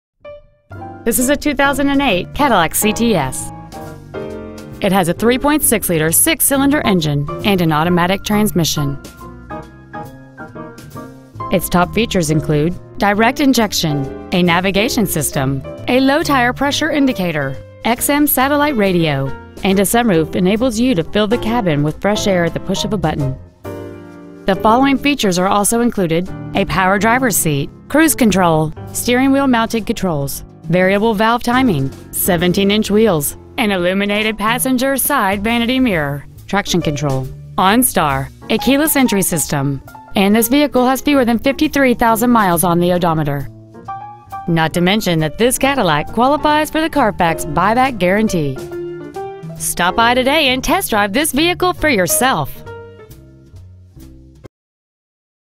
2008 Cadillac CTS Carrollton TX 75006
Year : 2008
 Make : Cadillac
 Model : CTS
 Engine : 3.6L VARIABLE VALVE TIMING V6 DI DIRECT INJECTION
 Trans . : Automatic
 Exterior : Red
 Miles : 52,814
 Interior : Black
 Stock : 12298A

 1875 N. Interstate 35E

 2008 Cadillac CTS Carrollton TX